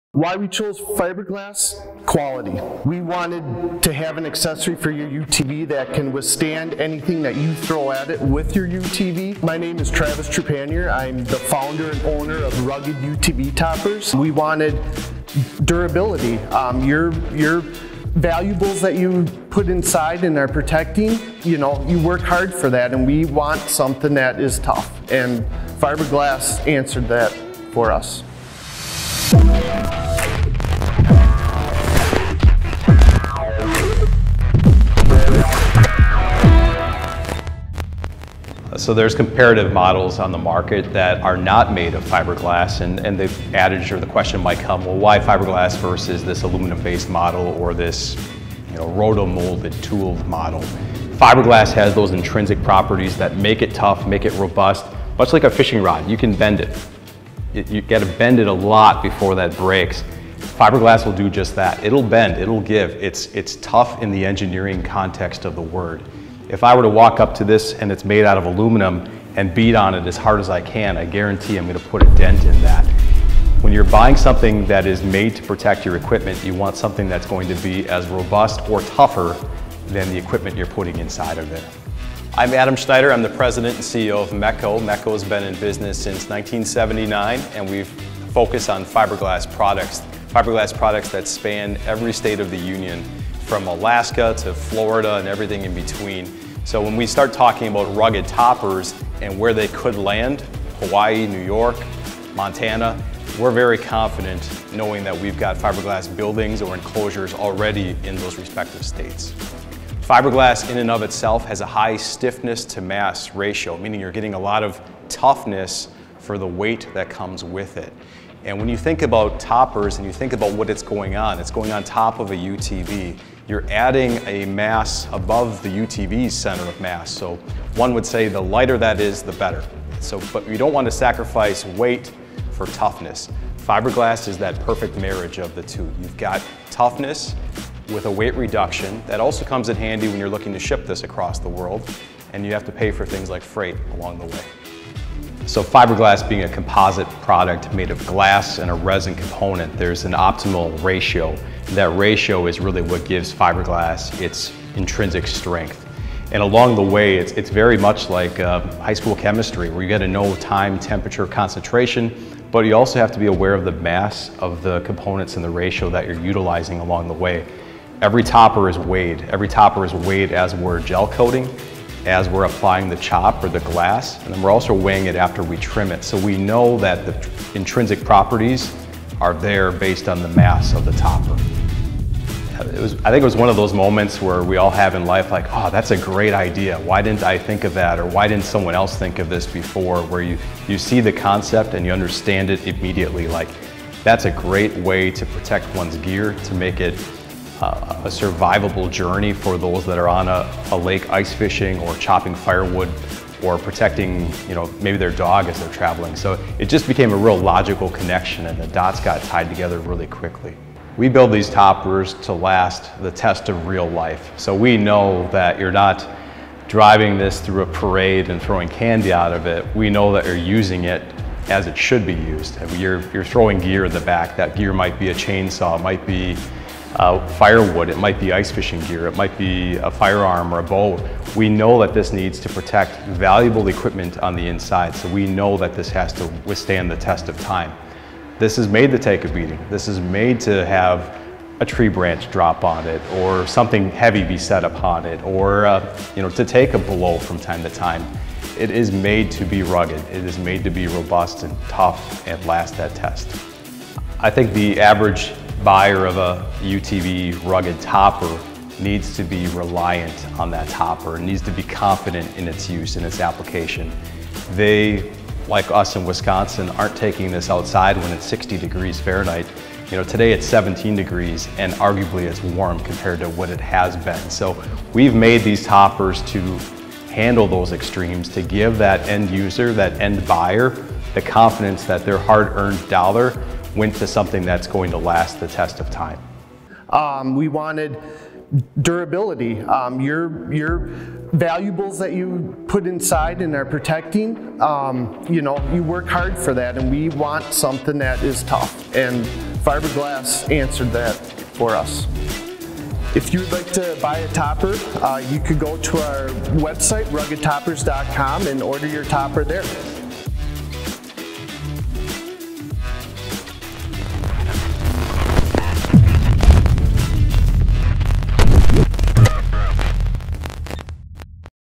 Why Fiberglass
The owner of Rugged Toppers, Travis Trepanier, and the President and CEO of MEKCO, Adam Schneider, team up to highlight their partnership and why fiberglass was chosen as the material of choice for Rugged UTV Toppers. During this process, we discover why fiberglass enclosures, like the Rugged UTV Topper, are a far superior material to other options on the market. The strength to weight ratio is unmatched allowing for Rugged's design to take high impact hits, extreme temperatures, and keep the center of gravity low in order to not stress the UTV's chassis.
Watch this video to better understand why Rugged choose fiberglass as their material of choice and why should as well.